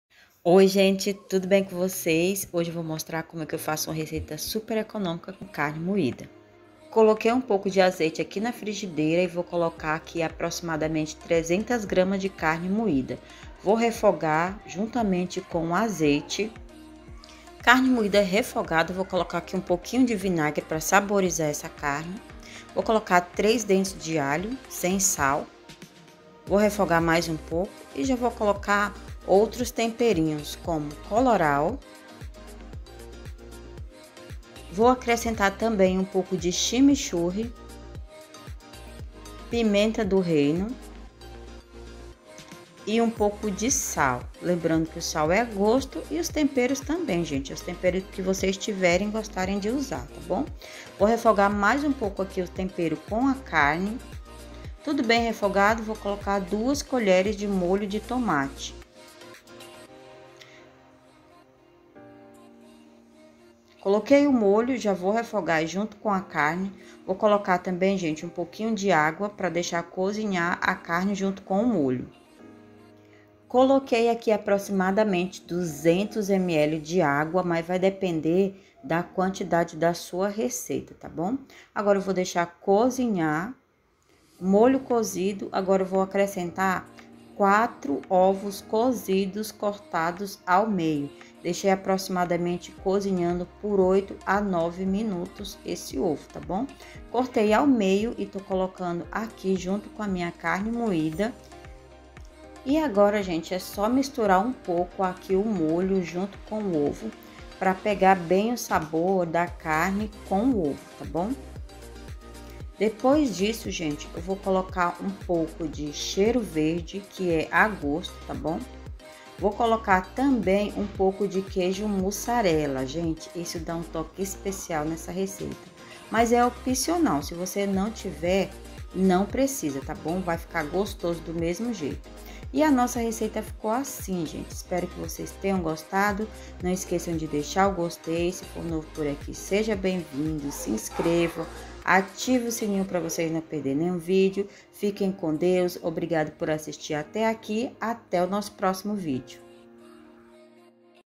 ECONOMIZE SUA CARNE COM ESSA RECEITA
Oi amores mostrei como faço mistura econômica pro almoço de carne moida com ovo espero que gostem acompanhem o vídeo passo a passo pra não perder nem um detalhe bjos até o próximo vídeo!!

claudia_receitas

ingredientes
300g de carne moida
4 ovos cozidos
2 colheres de extrato de tomate
3 dentes de alho
chimichurri
coloral
pimenta do reino
sal